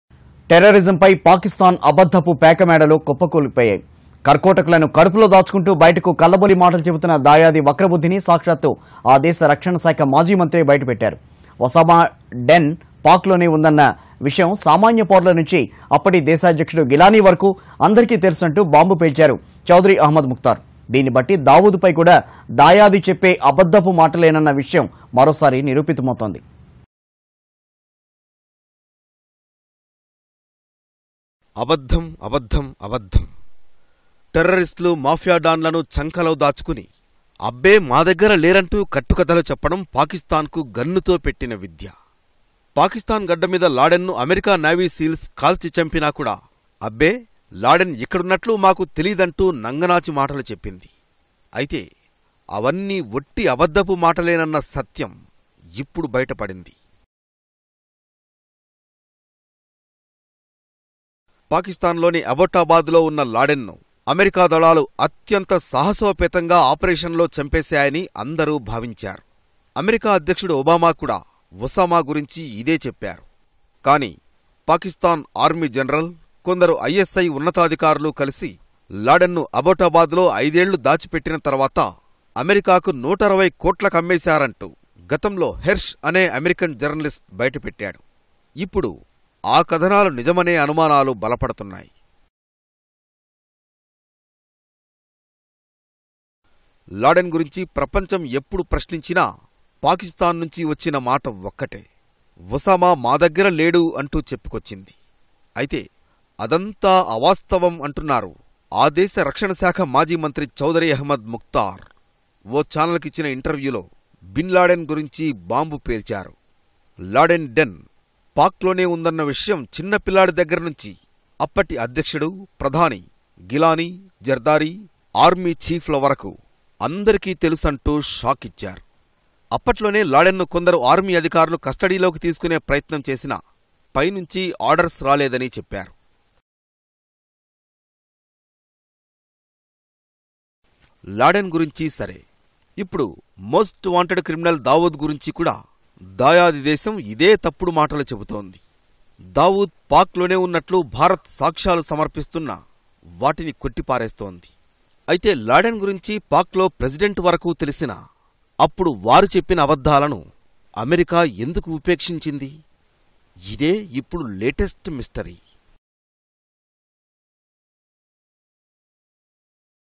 Top leaders knew about Osama Bin Laden's presence : Ex Pak Defence Minister - Express TV
Top leaders knew about Osama Bin Laden's presence : Ex Pak Defence Minister